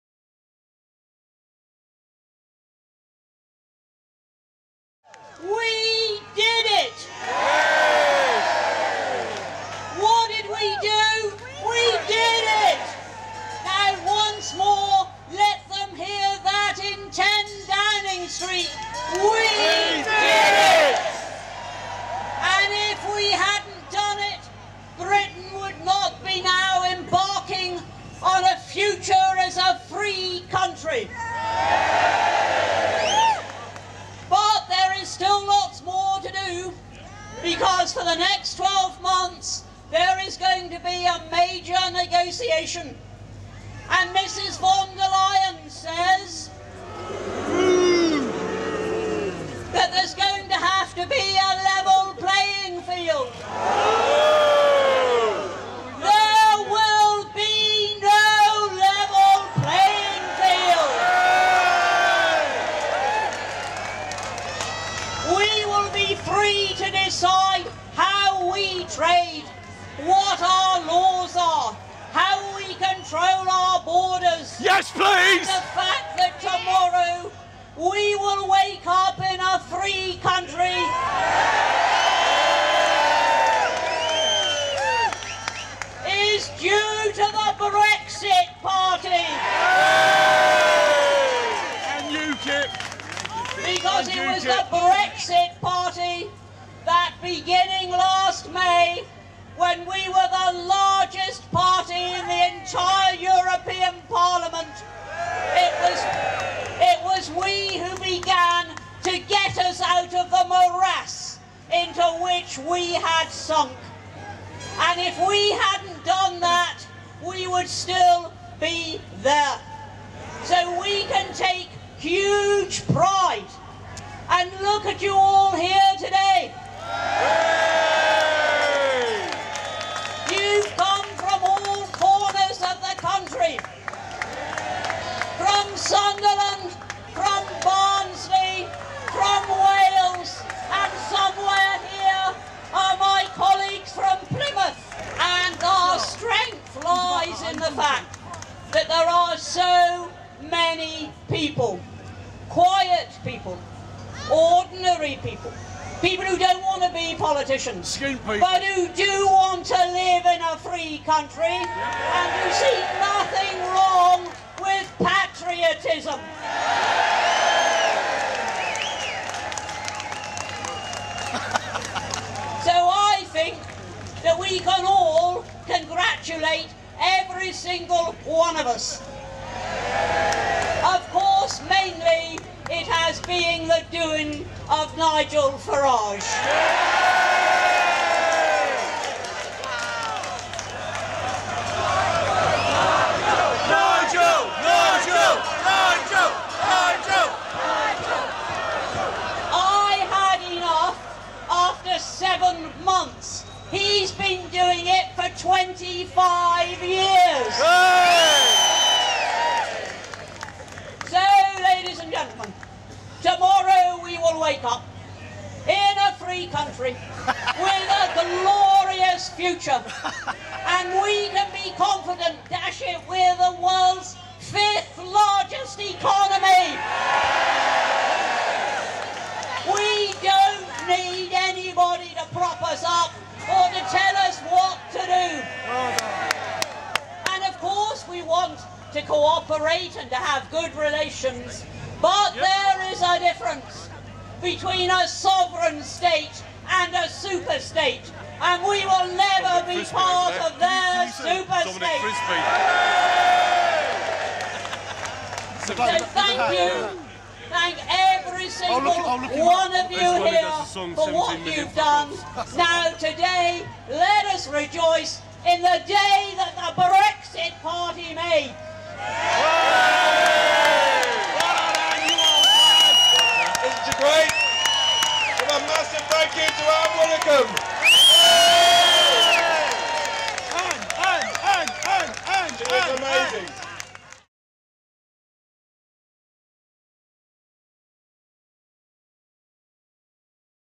Ann Widdecombe Speech on Brexit Day!
Ann Widdecombe gives a Rousing Speech on Brexit Day in front of packed crowd!